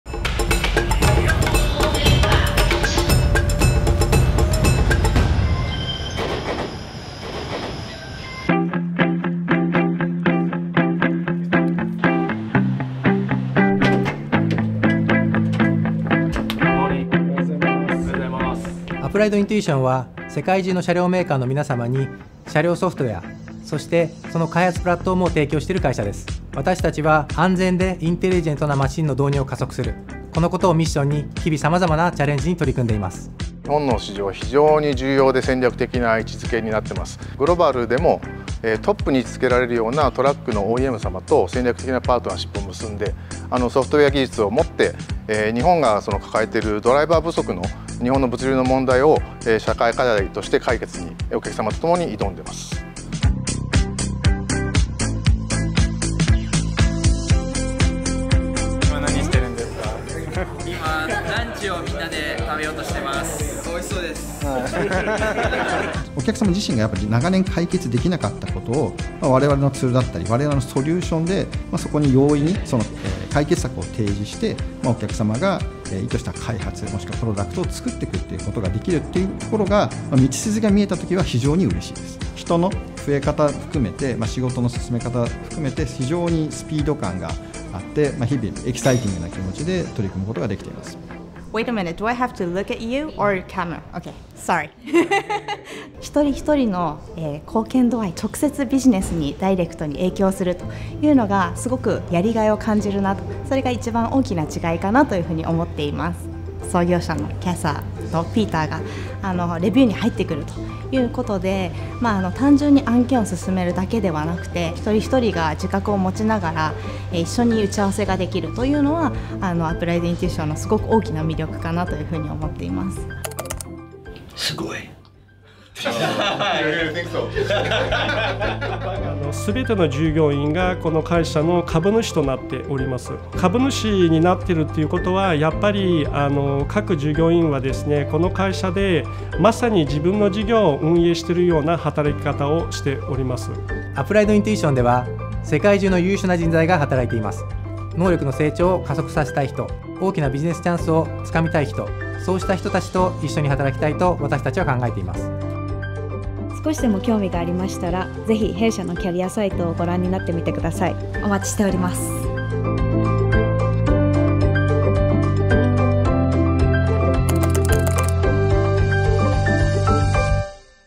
Life at Applied Intuition Japan
Applied Intuition は世の中に本当の価値と売上を出す本質的な仕事（リアルワーク）をするチームです。自動走行業界における最も困難な問題を解決し、安全でインテリジェントなマシンの世界的普及を一緒に加速しませんか？

✅ シリコンバレーの最先端技術でイノベーションを推進
✅ 世界トップ企業から集まるエンジニアリング人材
✅ 革命的なL4自動走行トラックが誕生への立ち会い